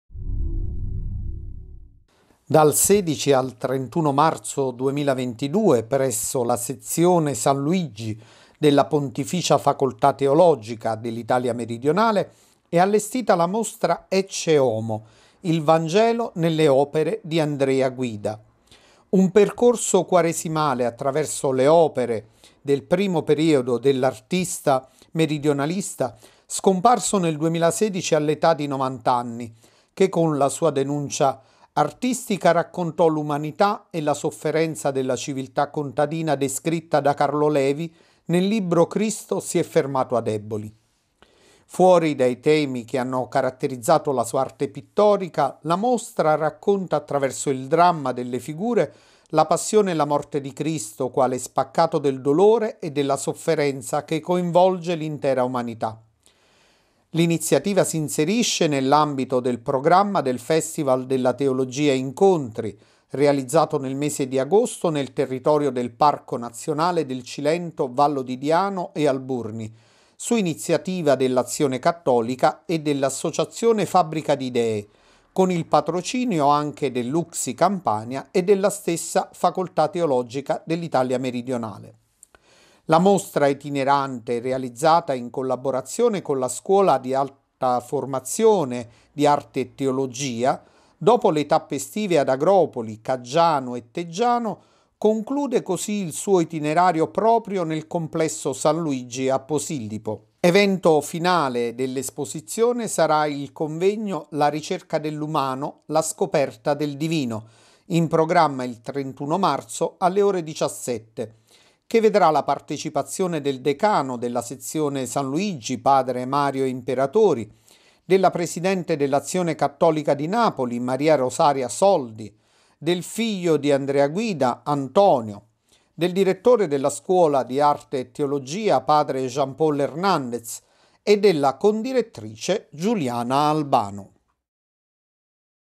Napoli, Il Vangelo di Andrea Guida in mostra a Posillipo
Dal 16 al 31 marzo 2022 presso la Sezione San Luigi della Pontificia Facoltà Teologica dell’Italia Meridionale è allestita la Mostra “Ecce Homo. Il Vangelo nelle opere di Andrea Guida”.